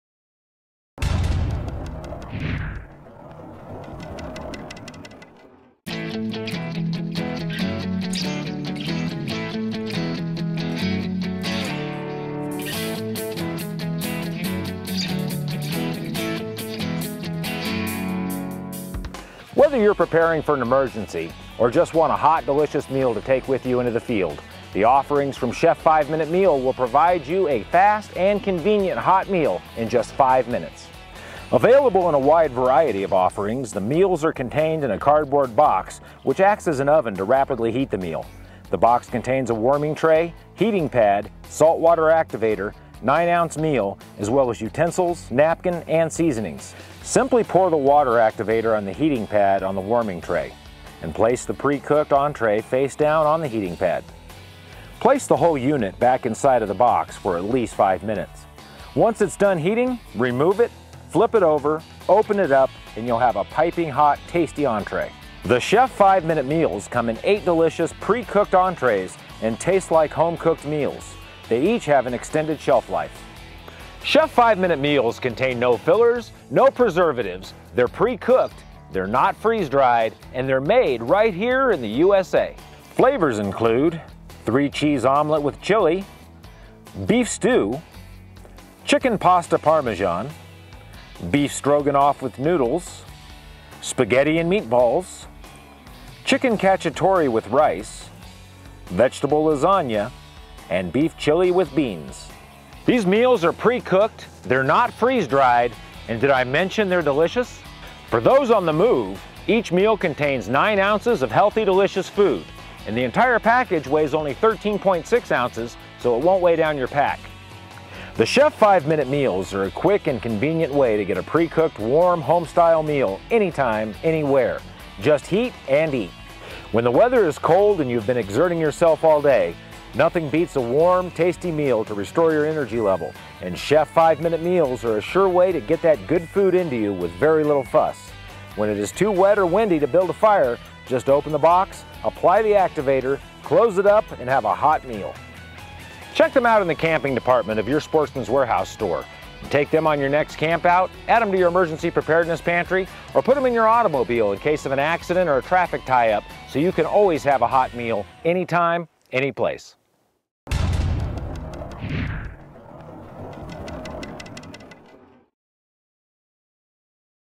Chef 5 Minutes Meals-- Video Review
Home-cooked taste, not freeze fried, delicious and quick hot meals for camping, emergency preparedness, and hiking. Fully cooked, just use the included warming tray for a piping hot meal any time, any place.